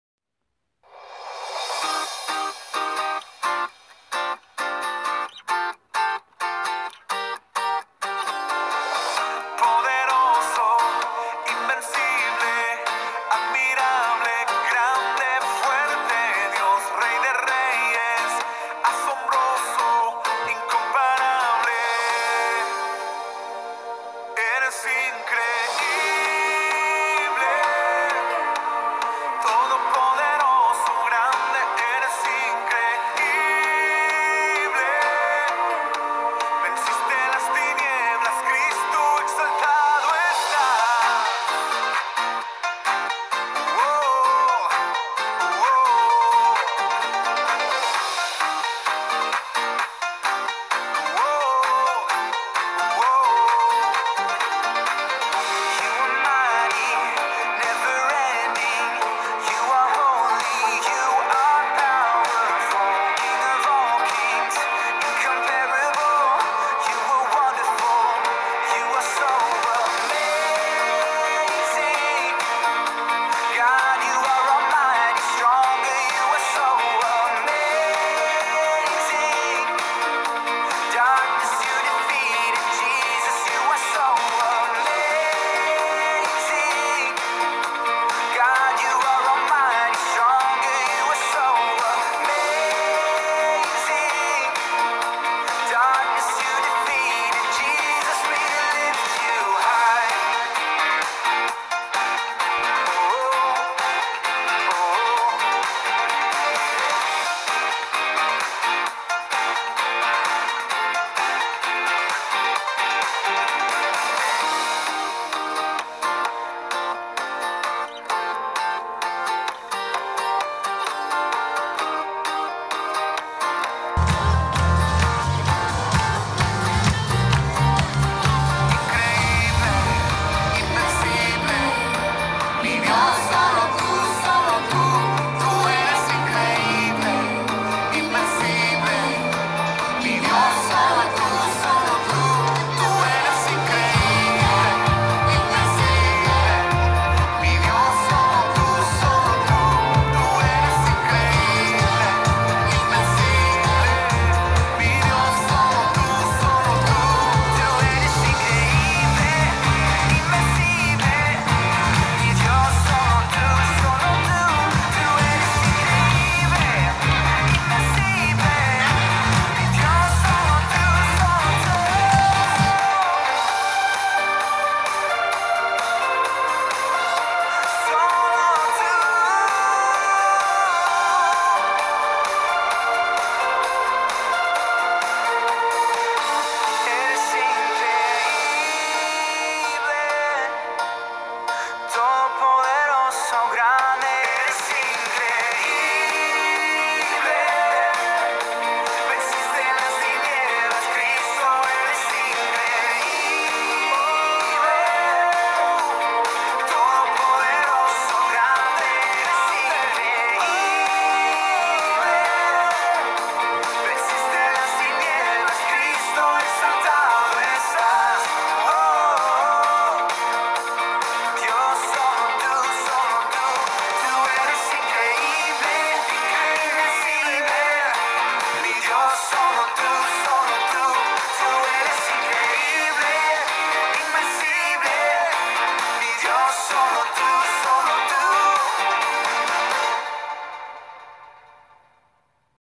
Mission trip with HFBC to Dominican Republic
Houston First Baptist Church's Mission team to Ramon Santana, Dominican Republic to serve International Freedom Ministries.